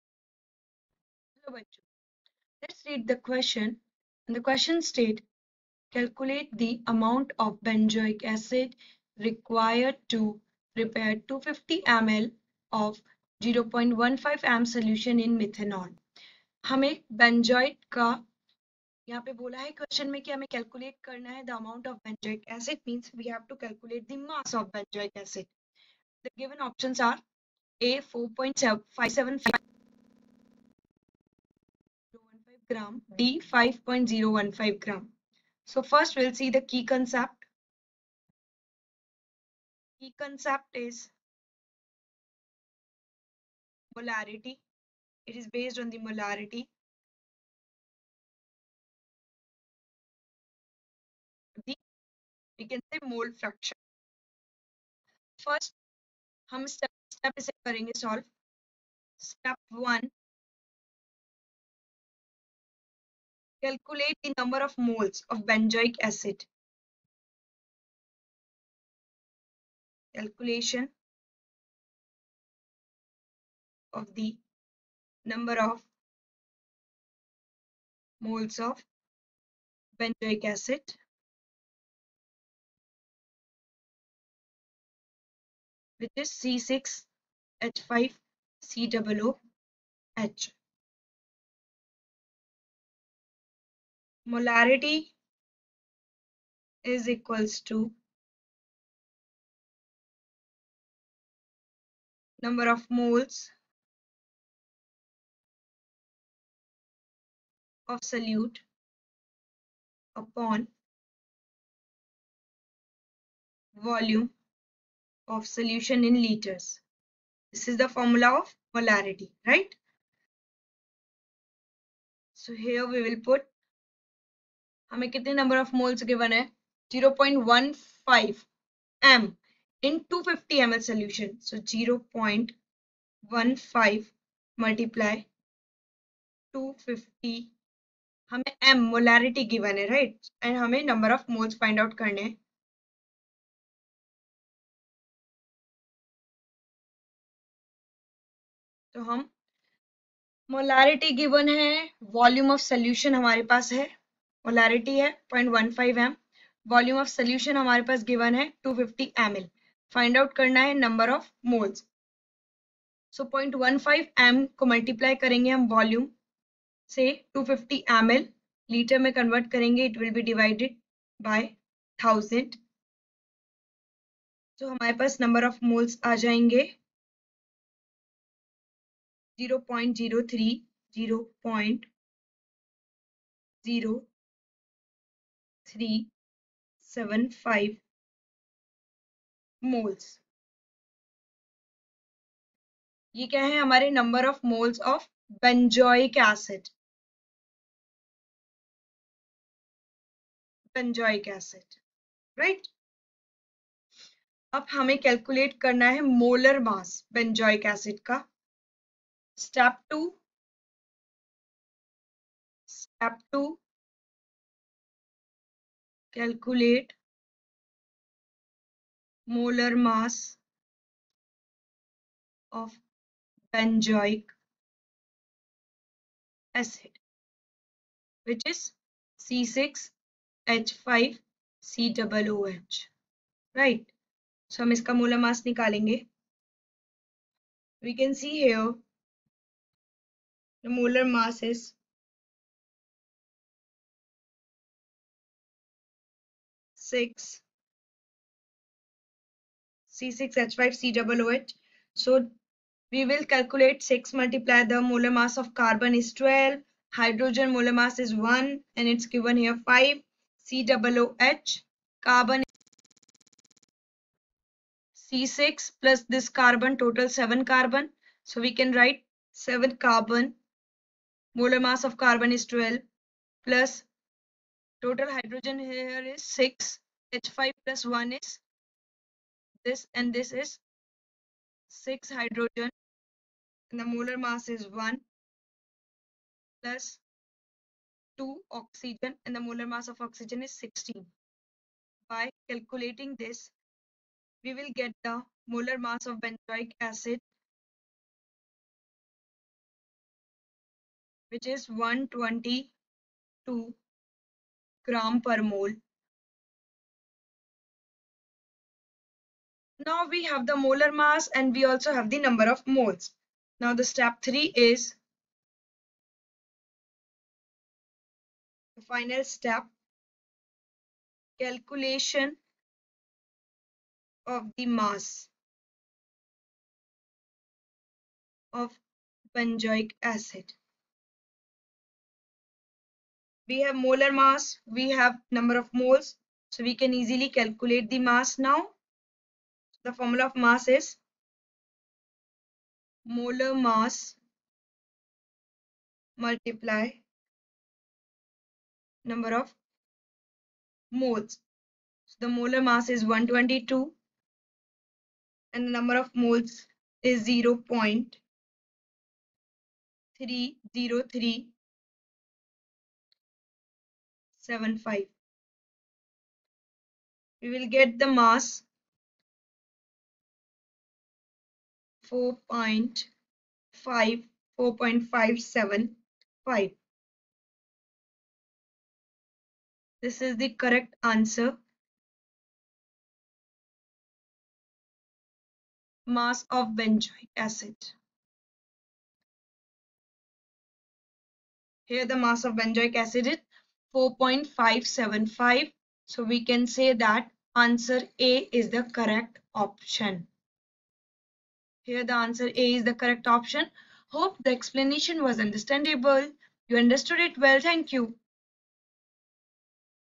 Calculate the amount of benzoic acid required to prepare \( 250 \mathrm{~mL} \) of \( 0.15 \math...
Calculate the amount of benzoic acid required to prepare \( 250 \mathrm{~mL} \) of \( 0.15 \mathrm{M} \) solution in methanol
(A) \( 4.575 \mathrm{~g} \)
(B) \( 5.475 \mathrm{~g} \)